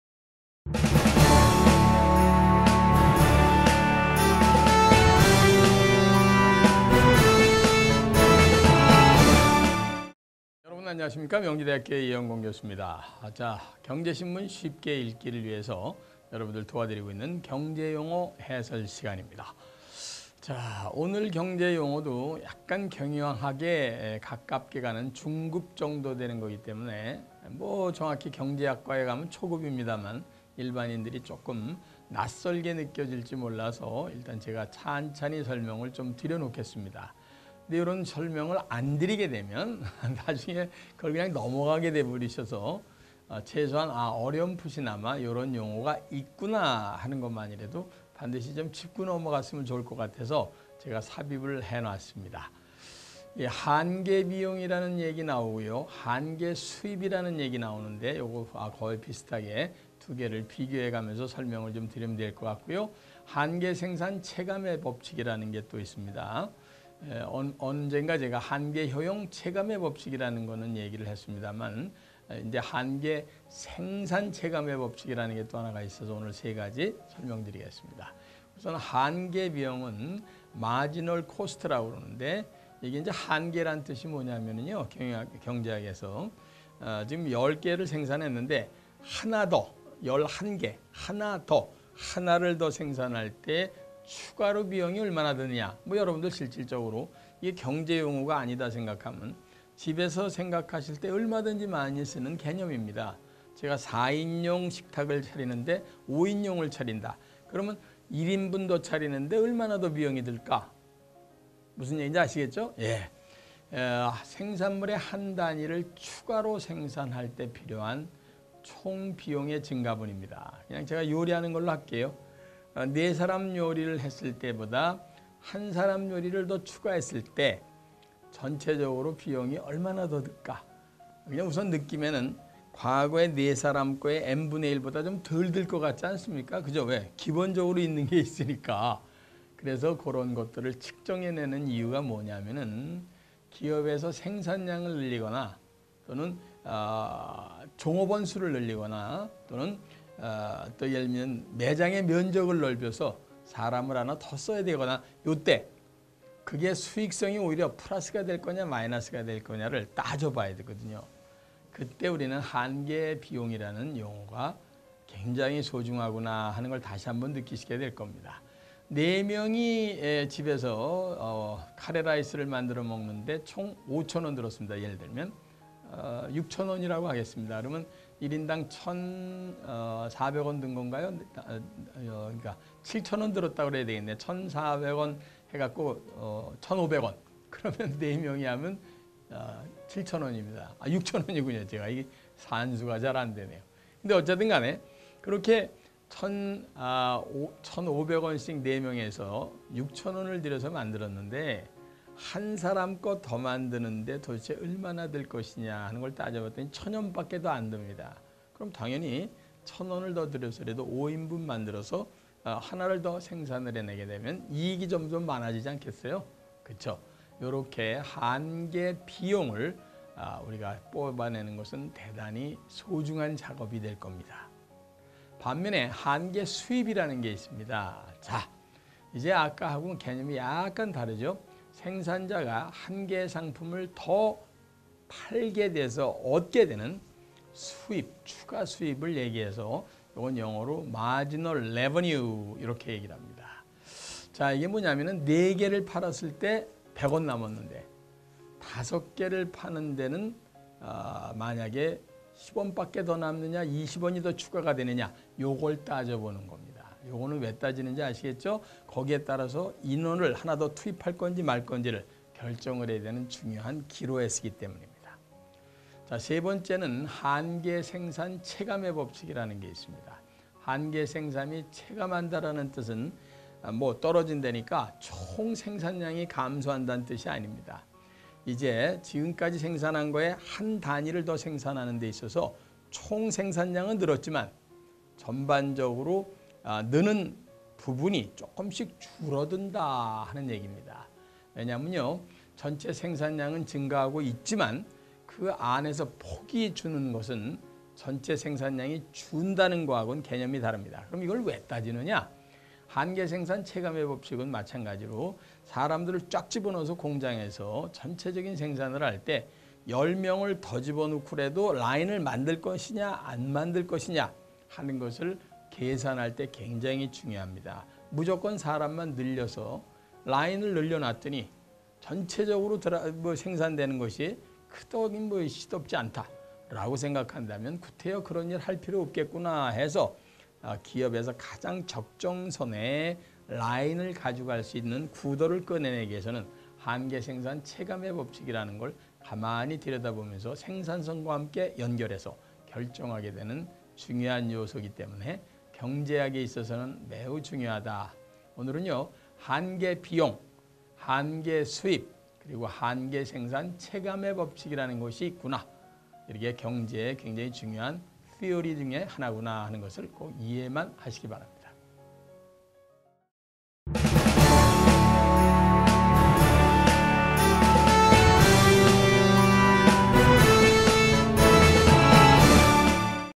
[경제신문 읽기] 117강.한계 생산체감의 법칙!
이영권의 경제신문 쉽게 읽기

117강.한계 생산체감의 법칙!

* 2012년 영상입입니다.
* [강연의 시대]는 대한민국의 소중한 강연자료들을 모아놓은 아카이브 채널입니다
* 본 영상은 오간지프로덕션과 (주)석세스티브이가 공동 제작한 강연입니다.
* 고 이영권 박사님의 명복을 빕니다